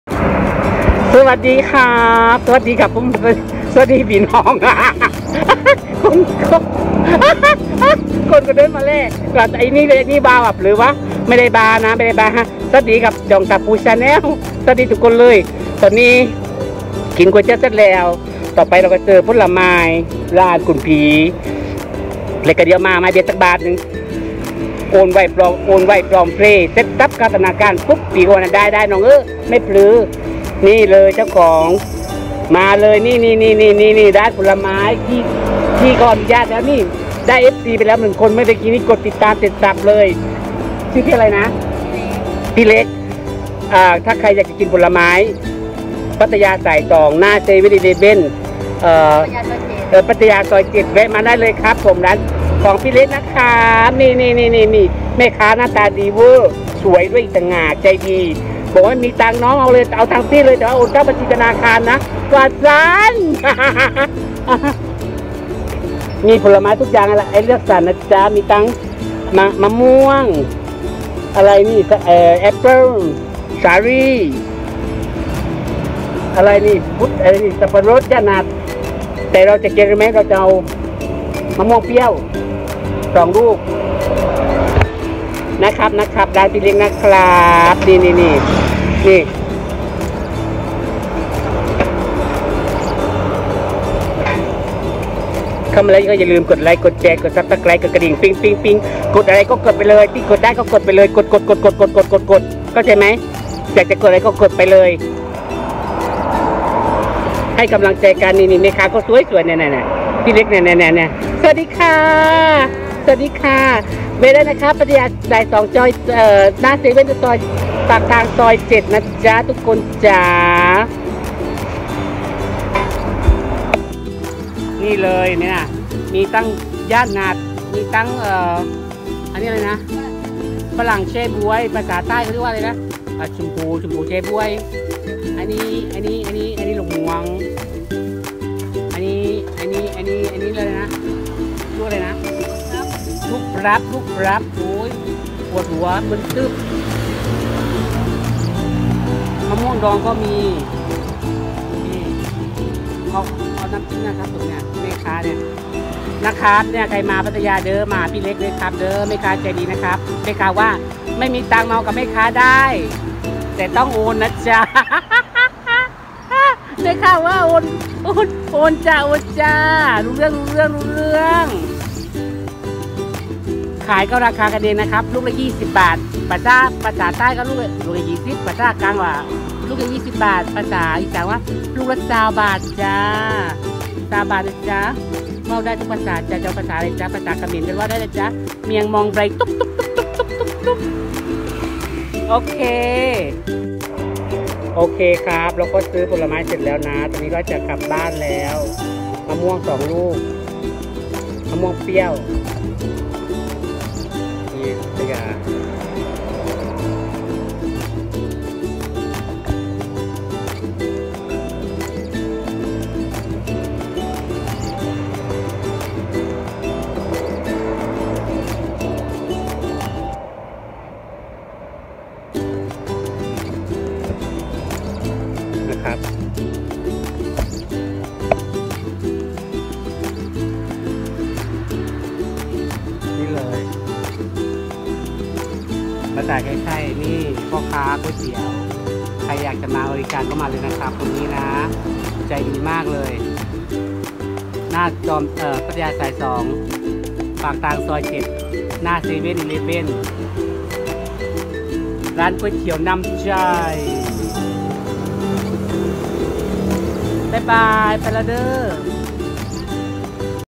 Ep130 มะม่วงเปรี้ยวเสียวสะท้านทรวง!มะม่วงเปรี้ยวซี๊ดซ๊าด
กินมะม่วงเปรี้ยวเข็ดฟันมาก